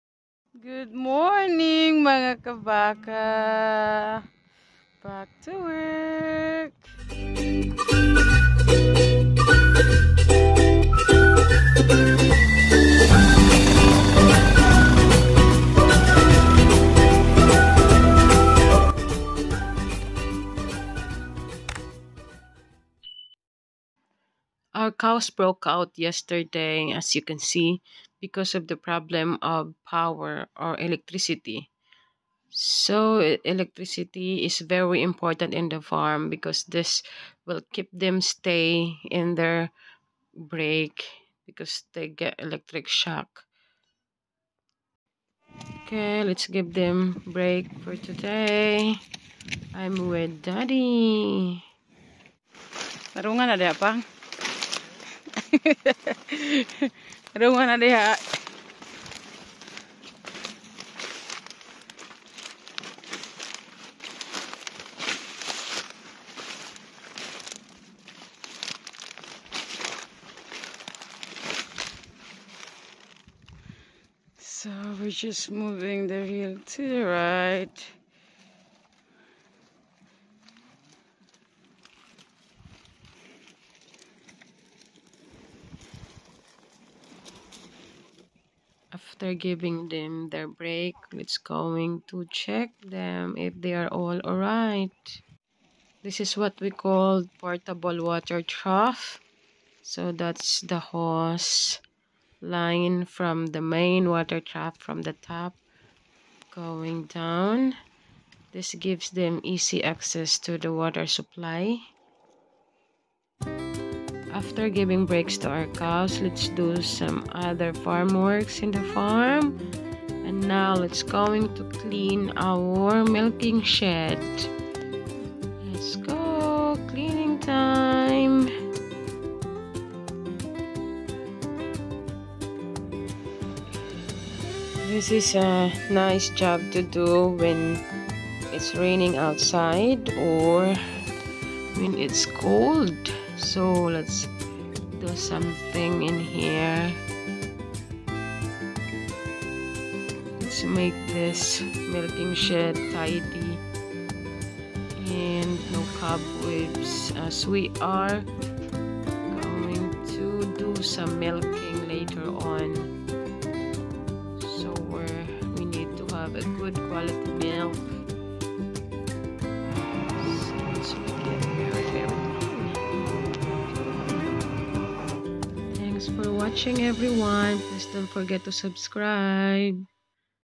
Dairy Farm Works - feeding + cleaning
feeding cow with foadder beet and cleaning shes